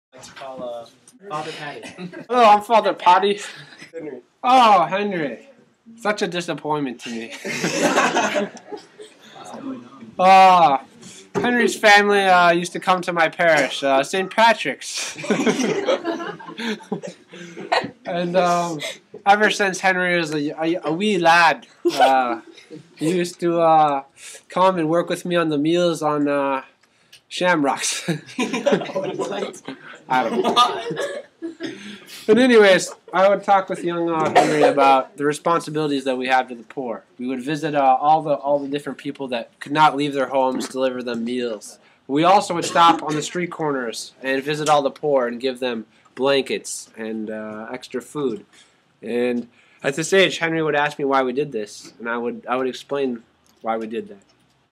Guerin Catholic Theology Class- "Father Patty" speaks of how he knows Henry
GCHS Theo. Class- Father Patty played by Mr. W. (Also Judge Benedict) tells the court how he knows Henry and what they used to do together.